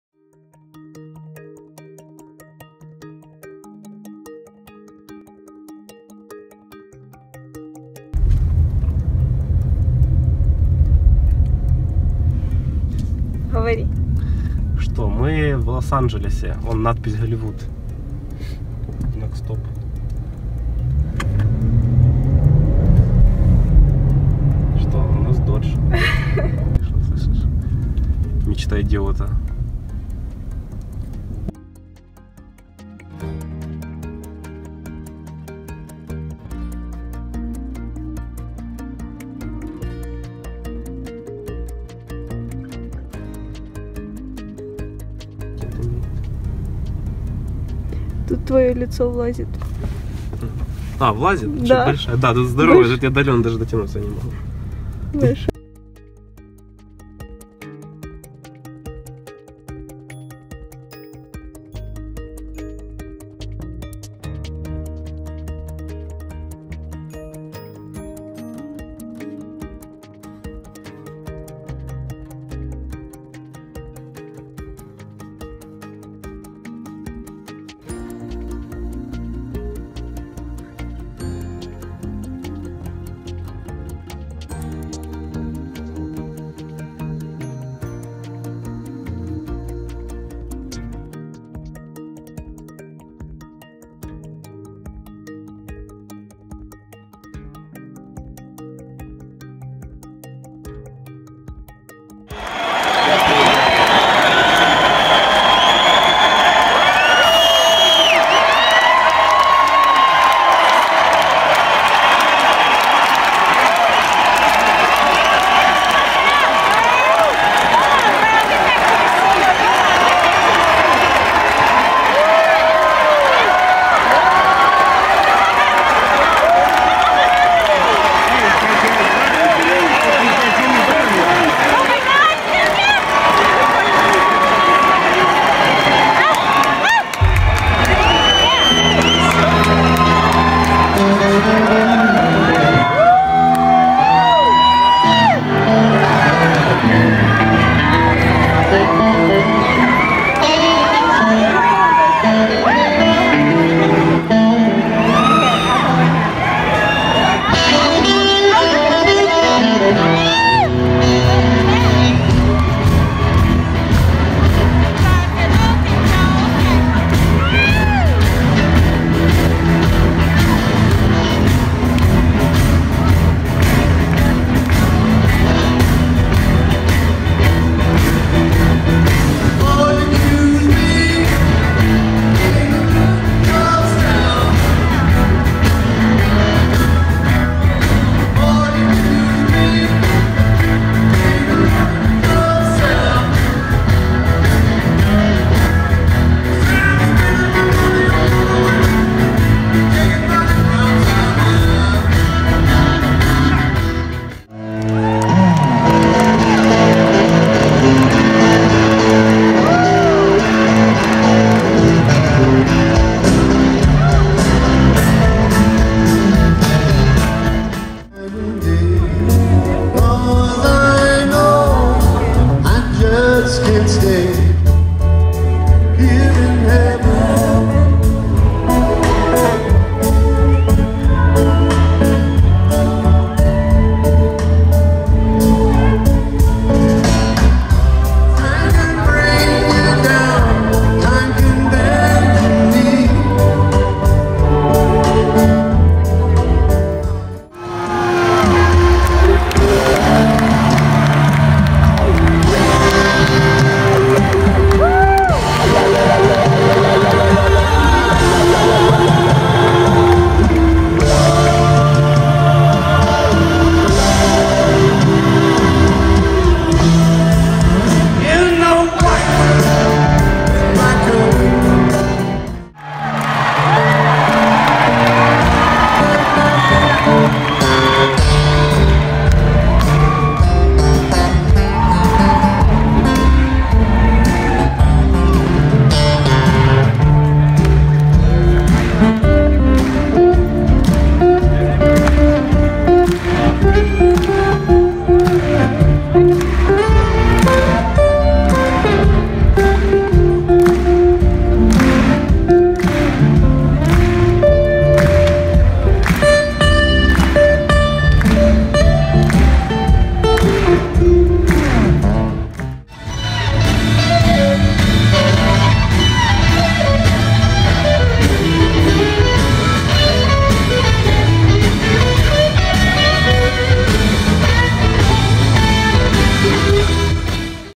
Roller Toaster Trip. Episode .21 / USA / LA / Hollywood
- летим в Лос-Анджелес
- катаемся по городу
- Голивудские холмы и обсерватория Гриффита
- аллея звезд
- концерт Эрика Клэптона